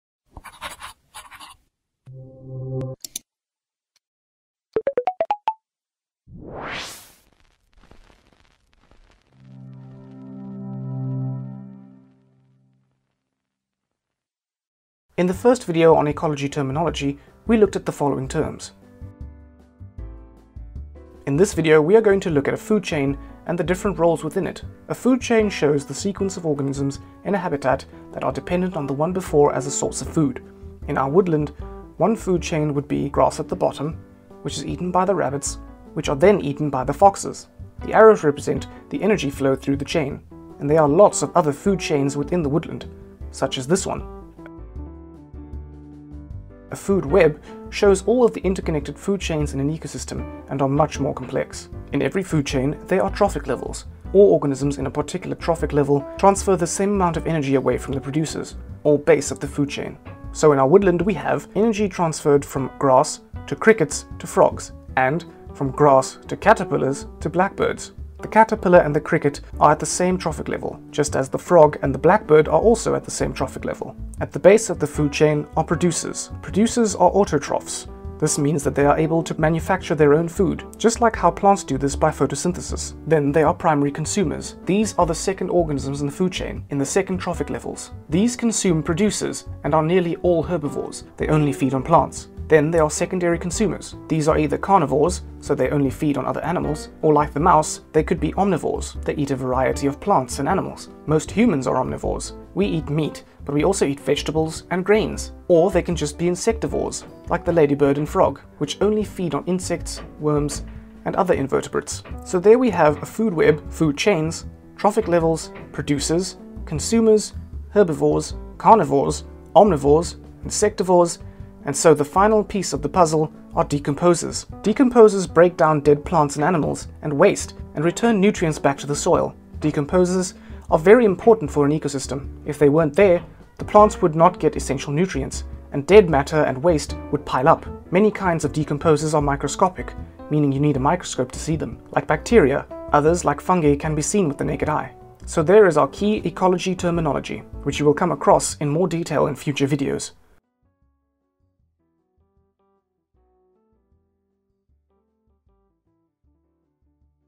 Food Chain | Ecology and Environment | Biology FuseSchool
In this video, we are going to look at a food chain and the different roles within that. So we will discover exactly what each of these words mean: food web, food chain, trophic level, producer, consumer, herbivore, carnivore, omnivore, insectivore, decomposer.

A food chain shows the sequence of organisms in a habitat that are dependent on the one before as a source of food. So in a woodland, one food chain would be grass at the bottom, which is eaten by rabbits, which are then eaten by foxes. The arrows represent the energy flow through the chain.

A food web shows all of the interconnected food chains in an ecosystem, and are much more complex.

In every food chain, there are trophic levels. All organisms in a particular trophic level are the same number of energy transfers away from the producers - or base of the food chain.

At the base of all food chains are producers. Producers are autotrophs. This means they are able to manufacture their own food. Usually plants by photosynthesis.

Primary consumers are the second organisms in the food chain, in the second trophic levels. These consume producers and are nearly all herbivores - they only feed on plants.

Then there are secondary consumers. These are either carnivores - so they only feed on other animals. Or like the mouse, they could be omnivores. They eat a variety of plants and animals. Most humans are omnivores: we eat meat but we also eat vegetables and grains. Or they could just be insectivores, like the ladybird and frog. They only feed on insects, worms and other invertebrates.

Decomposers break down dead plants and animals, and waste, and return nutrients back to the soil. Decomposers are very important for any ecosystem. If they weren't there, the plants would not get essential nutrients, and dead matter and waste would pile up. Many kinds of decomposers are microscopic, meaning you need a microscope to see them - like bacteria. Others, like fungi, can be seen.

Ecology: Producer Consumer Herbivore Carnivore Omnivore Insectivore Decomposer